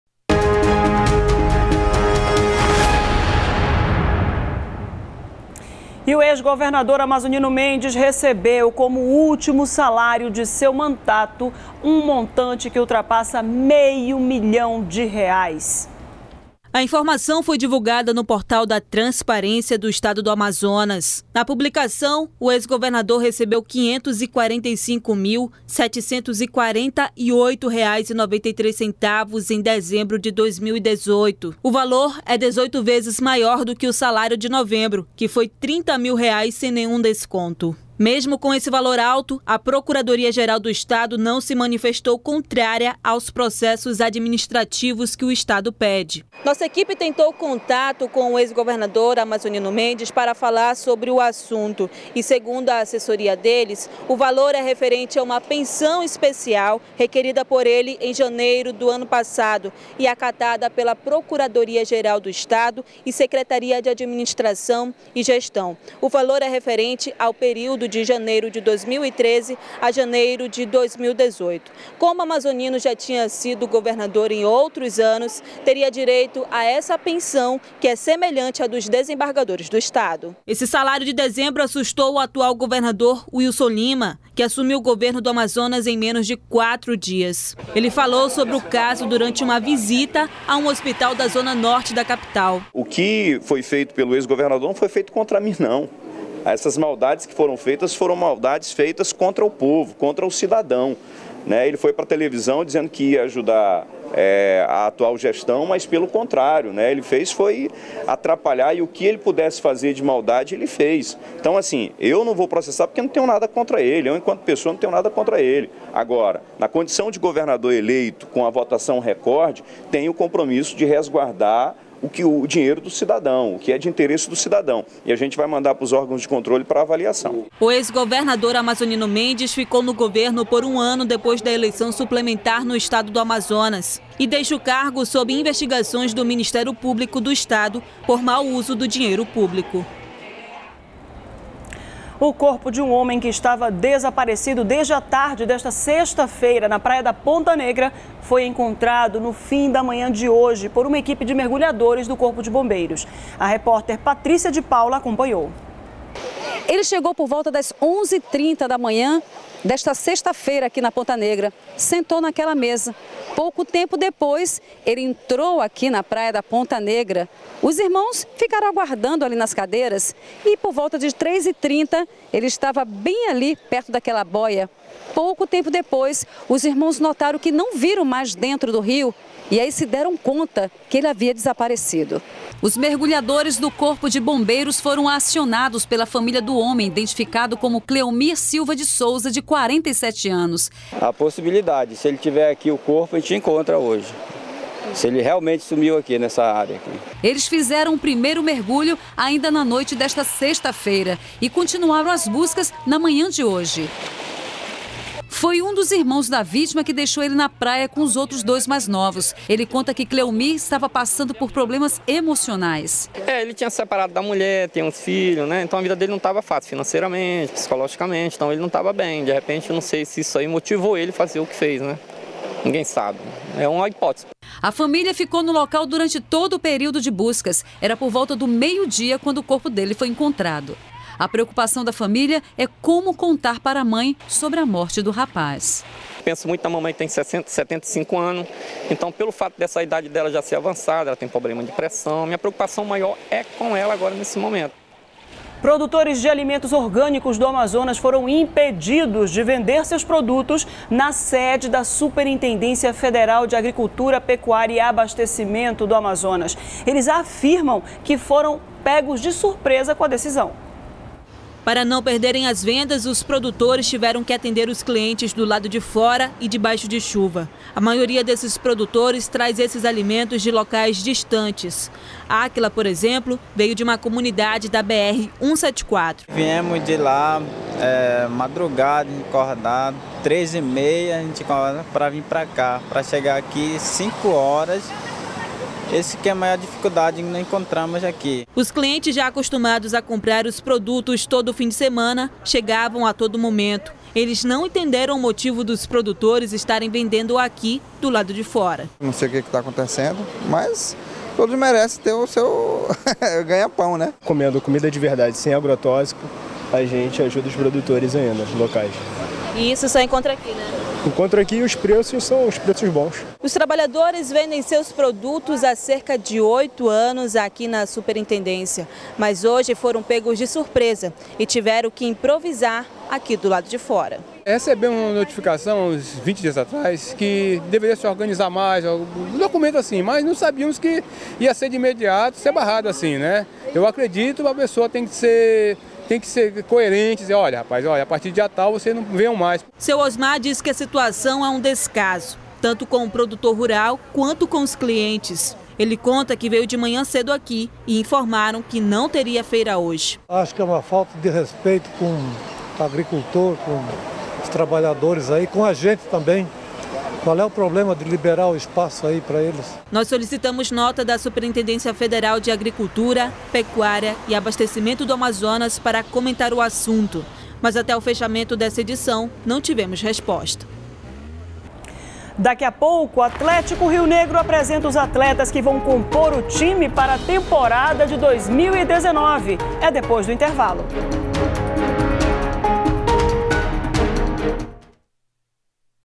Amazonino Mendes recebe mais de meio milhão de reais como último salário de dezembro
02º BLOCO JORNAL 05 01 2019 - Amazonino Mendes recebe mais de meio milhão de reais como último salário de dezembro